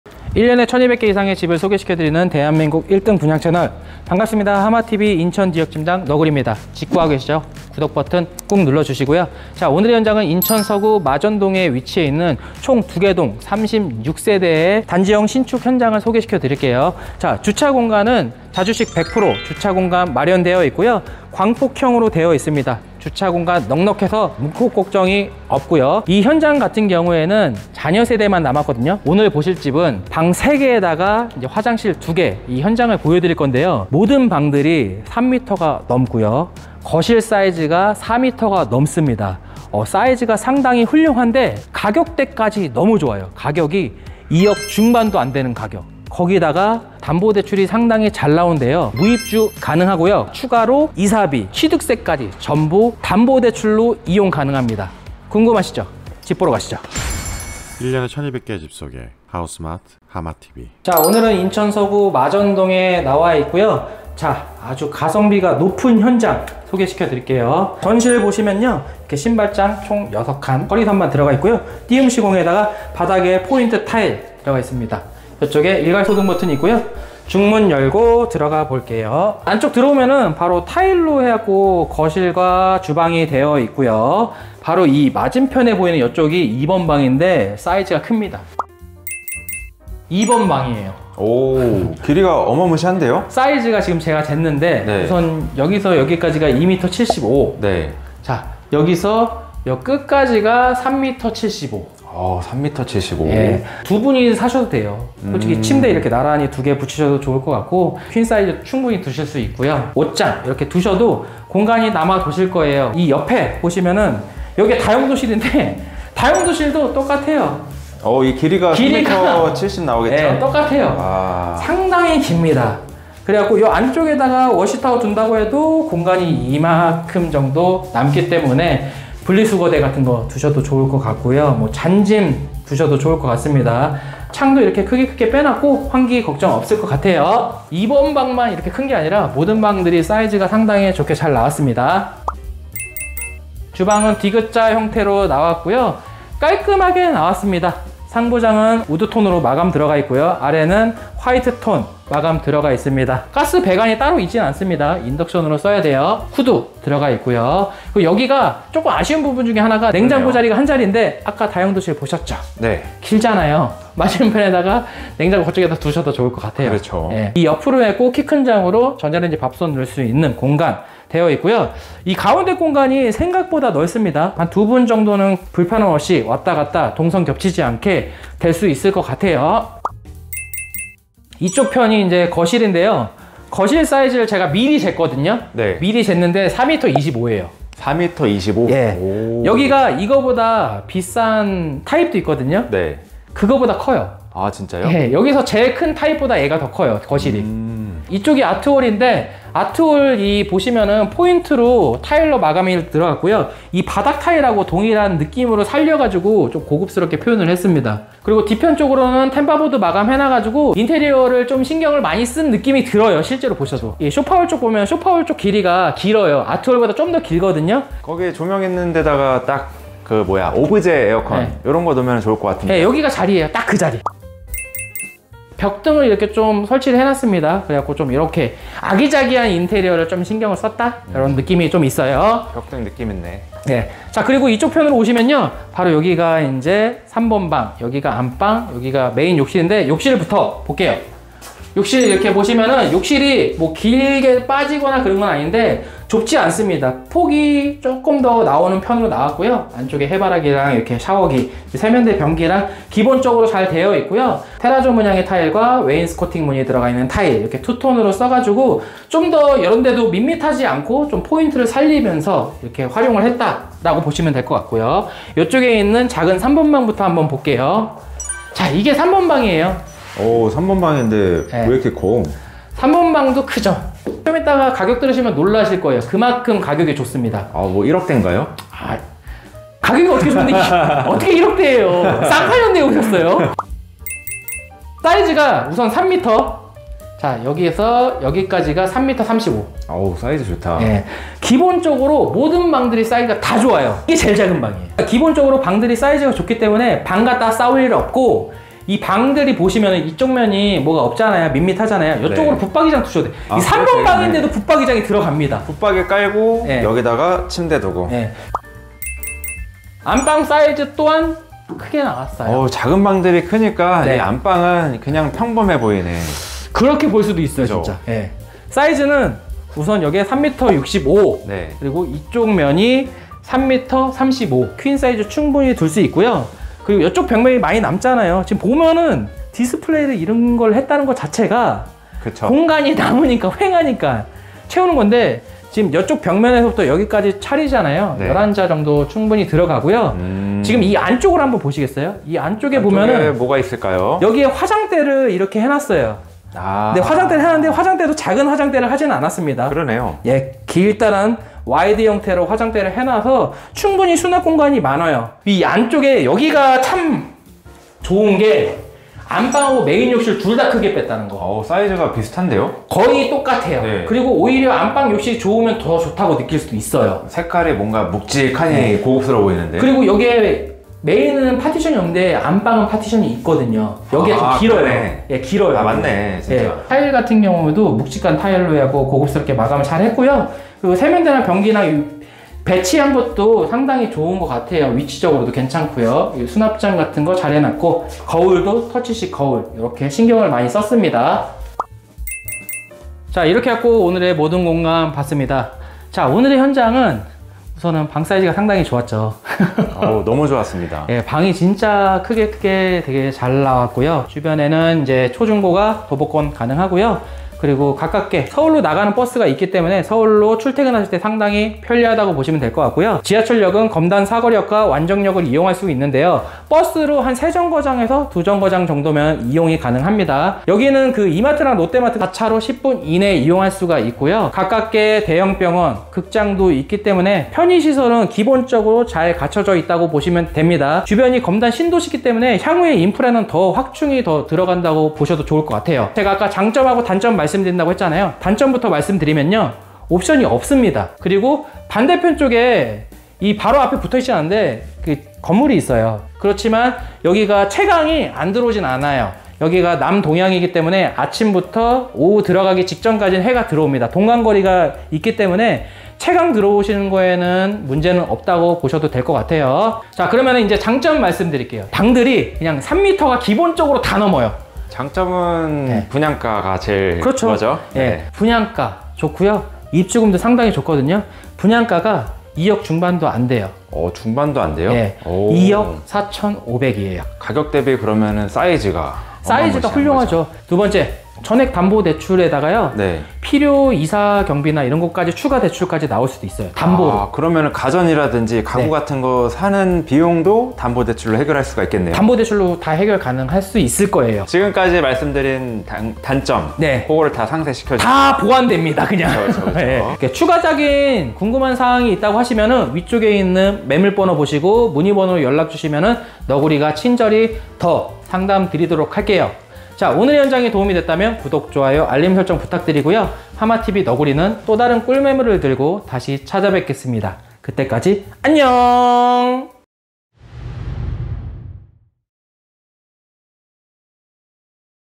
※전액대출※대형빌라 마지막 잔여세대! 계약금만 가져오세요 / 6651
안녕하세요
하마TV의 너구리 입니다

인천 서구 마전동 에 위치한 2개동 36세대 빌라입니다

주변 인프라
초중고 도보 10분
이마트 자차 5분
검단ic 자차 10분
검단 사거리역 완정역 자차 5분

방3 화2 다용도실1
인천 서구 마전동 3룸 현장입니다
분양가: 2억 중반
실입주금: 무입가능